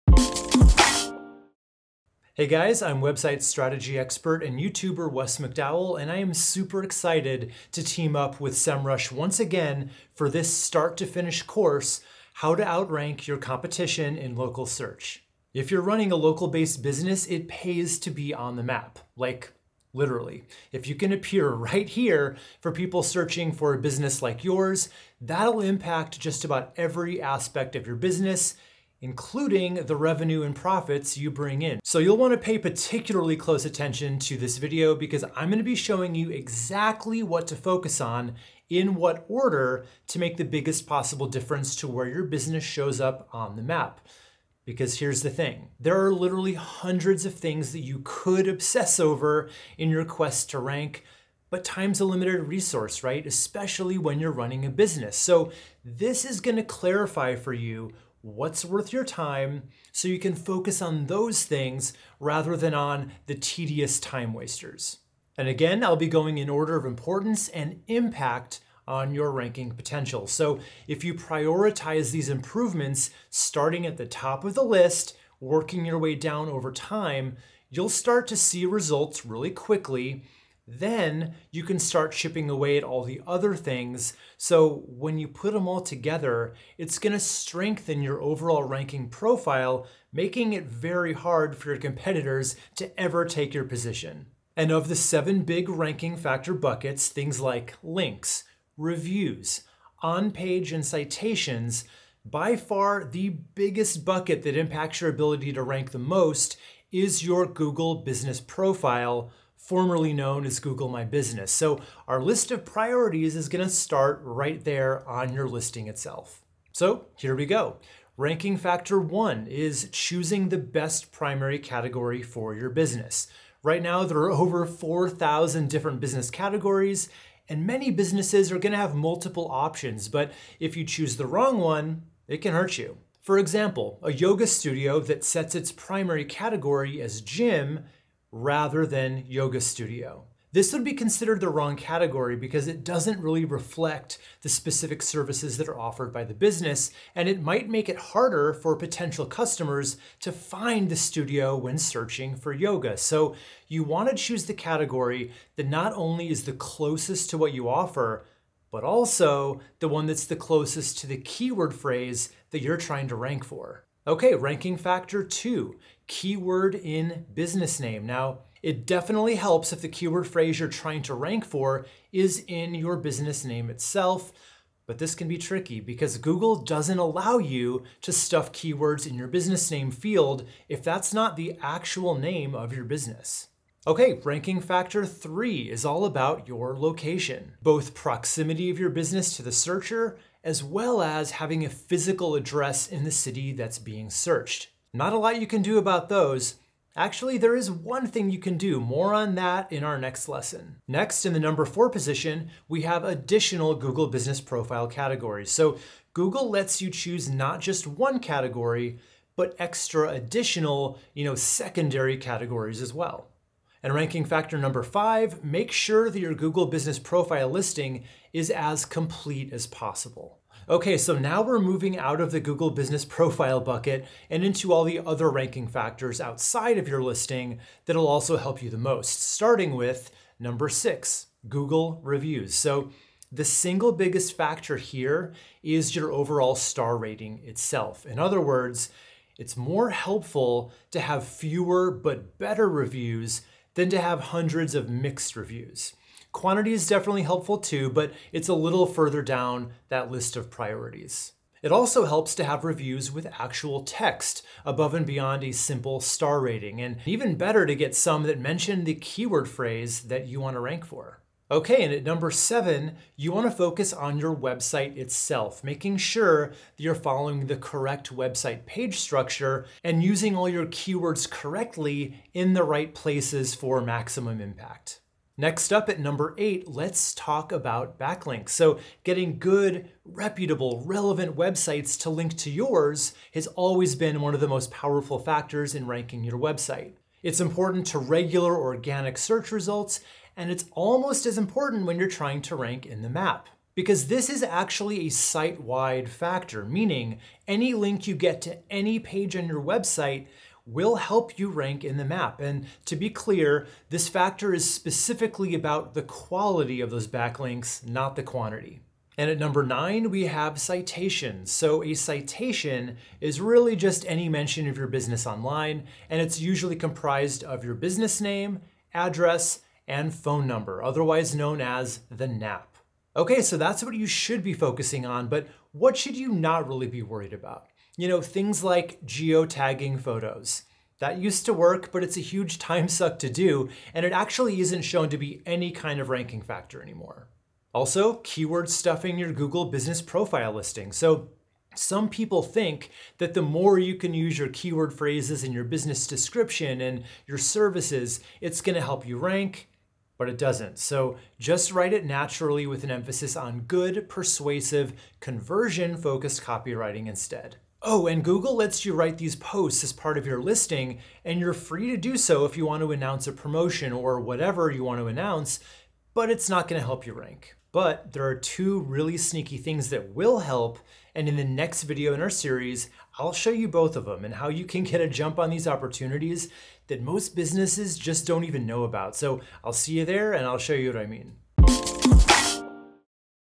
EXPERT EXPLAINS: Local SEO Ranking Factors
Wondering how you can show up on Google’s Local Map Pack? You're in the right place! Get to know the local ranking factors and start seeing results. 🎯 Enroll in the full course for free here ▶︎

⏱️In less than 7 minutes you’ll know exactly what improvements to prioritize so you can start seeing results faster.

NEXT STEPS:
1️⃣Be sure to watch until the end so you get to know all seven of the big ranking factor buckets
4️⃣If you haven’t already, enroll in the complete course which covers everything you need to know to outrank your competitors in local search. (It’s 100% FREE) ▶︎

The opinions of the speakers are his/her own and might not match the opinions of Semrush.